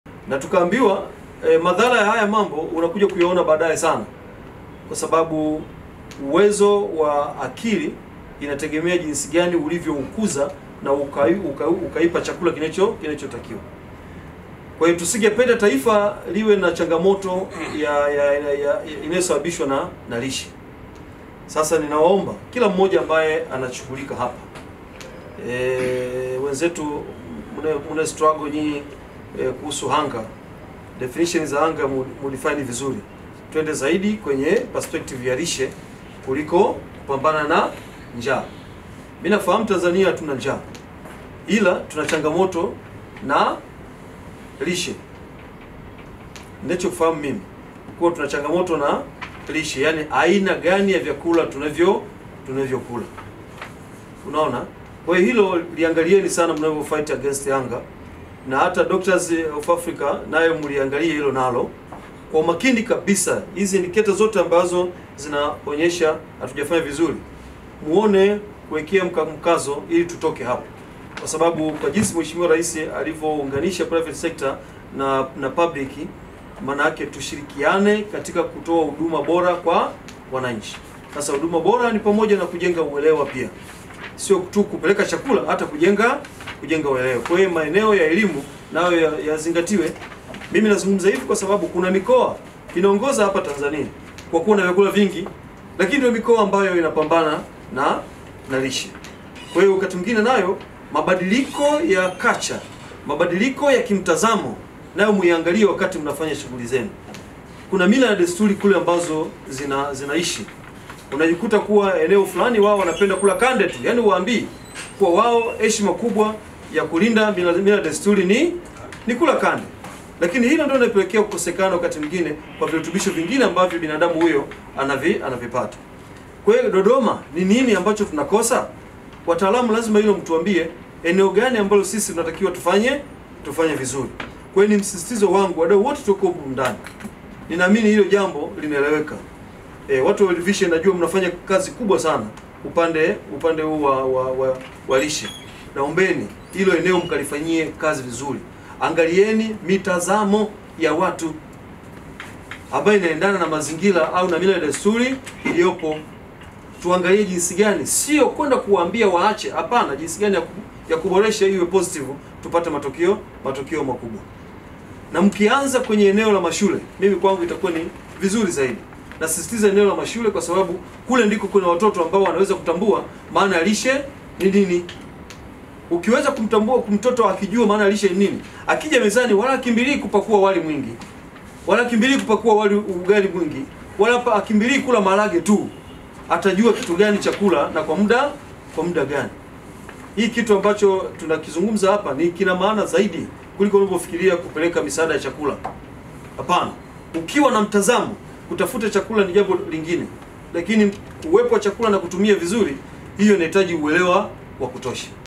'WANANCHI WAPEWE ELIMU YA LISHE' - RAS DODOMA
Katibu Tawala Mkoa wa Dodoma Bw. Kaspar Mmuya ameongoza kikao cha kamati ya lishe  robo ya kwanza (Julai -Septemba 2024) kujadili utekelezaji wa viashiria vya Mkataba wa Lishe.

Kikao kazi hicho kimefanyika Oktoba 23,2024 katika ukumbi wa mikutano wa Ofisi ya Mkuu wa Mkoa Jengo la Mkapa, ambapo ametumia fursa hiyo kuwaomba wadau na mashirika yanayojihusisha  na lishe  kutoa elimu kwa Wananchi ili kuepukana na desturi za kuamini katika chakula cha aina moja.